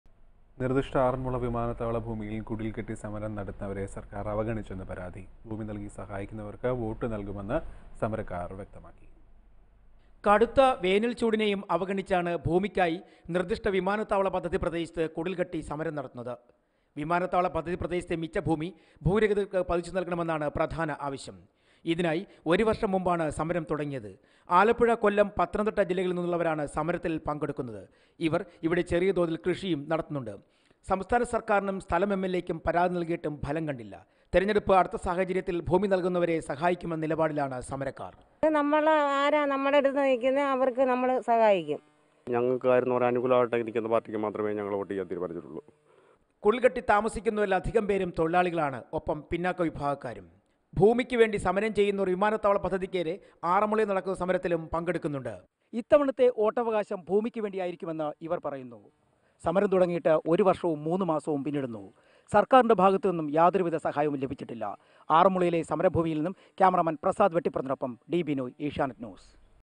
Land Issue in Aranmula
Landless people struggle in Aranmula, Pathanamthitta.